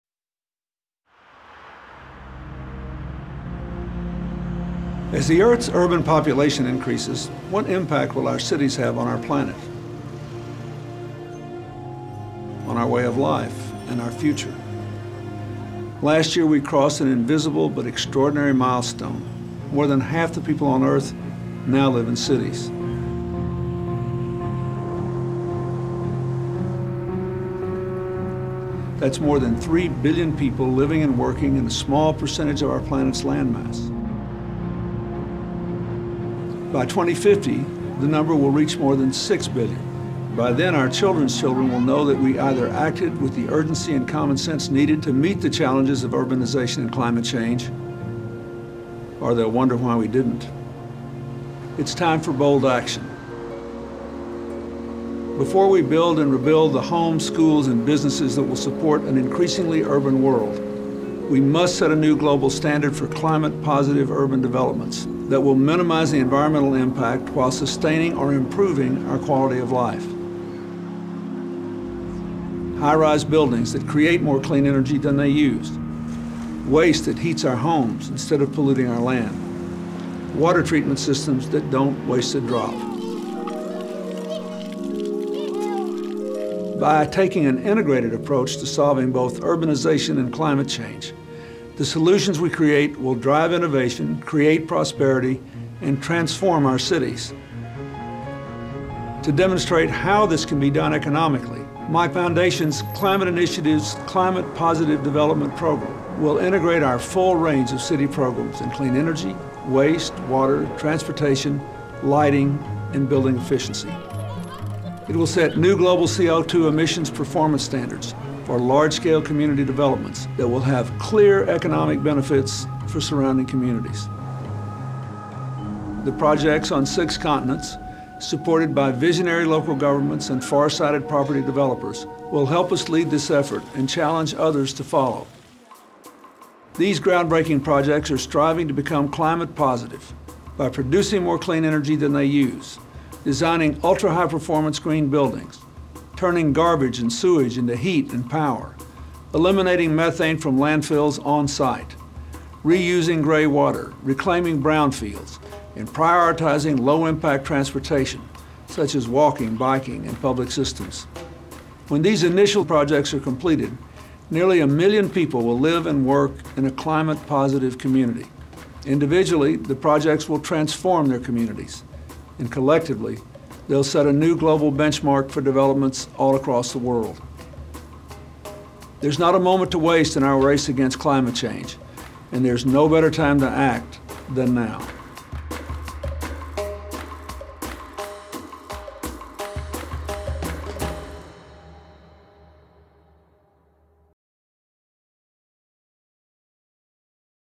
Cities Go Climate Positive
With the worlds population on the rise and more people moving to cities, President Clinton describes a bold new project that will set the global standard for communities striving to reduce greenhouse gas emissions to below zero.